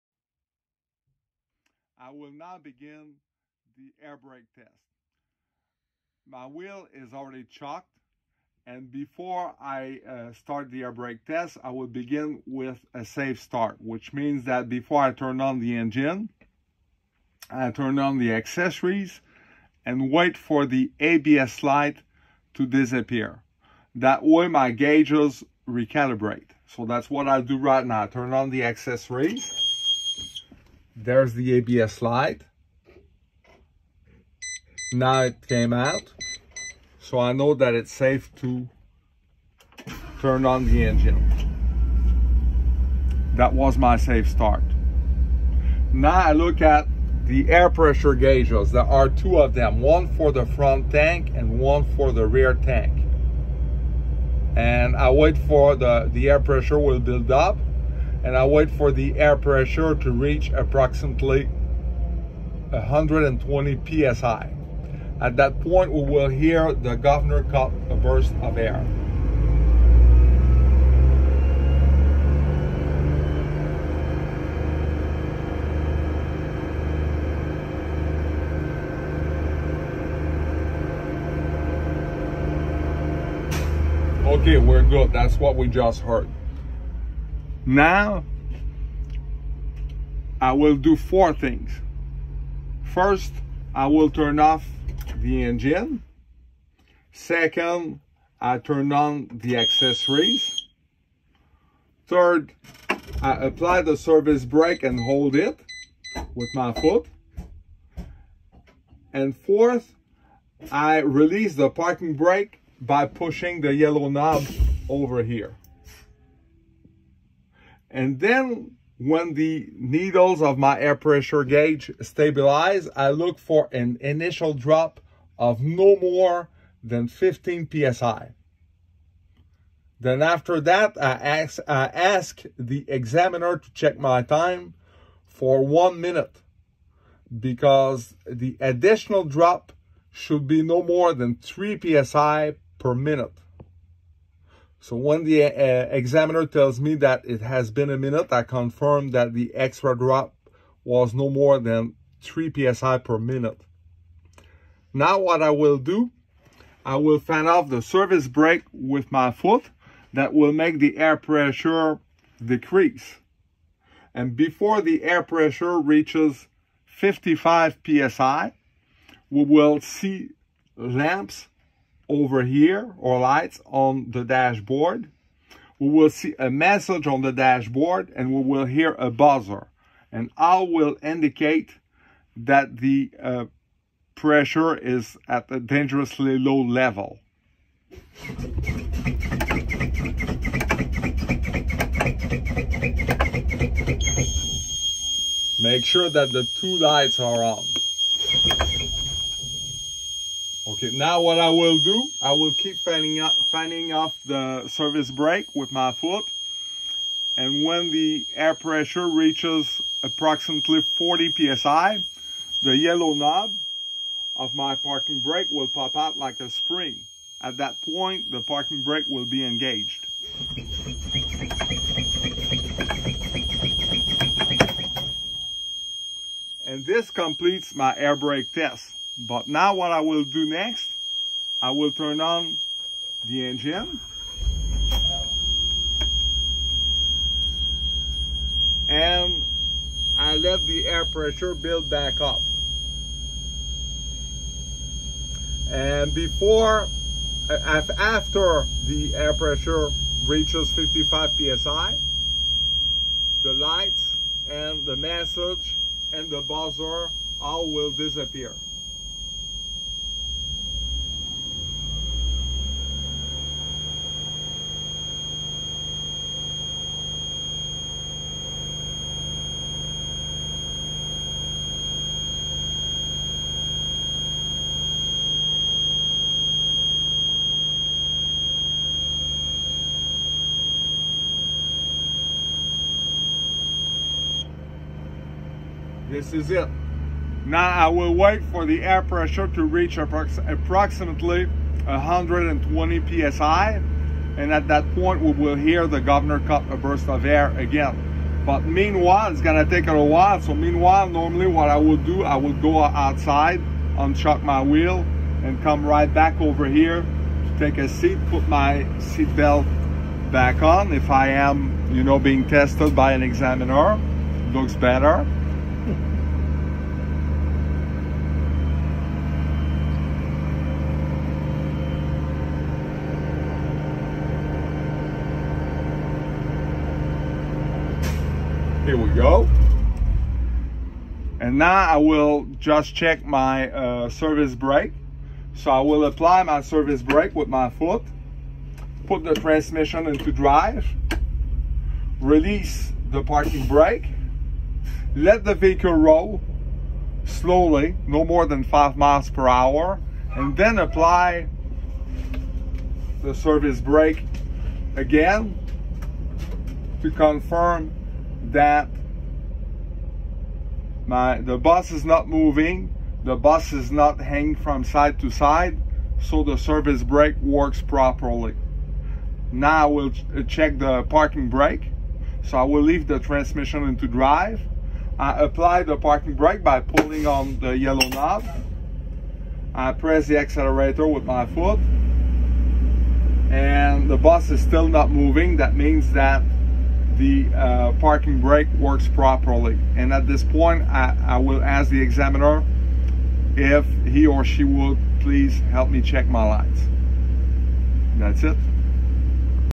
34. Air Brake Test – School Bus – Class B CDL
Presentation of the Air Brake Test on a 2022 Conventional Style BlueBird Vision School Bus in Richmond Hill, Georgia.

The numbers you must memorize (if you test in the state of Georgia) are:

Air pressure must reach approx. 120 psi (burst of air)
Initial drop: no more than 15 psi
Additional drop: no more than 3 psi / min.
Low air pressure warning signal: occurs before air pressure reaches 55 psi
Spring brake / parking brake engage: occurs at approx. 40 psi

For more details, refer to your state's Department of Motor Vehicles-issued Commercial Driver's License Manual.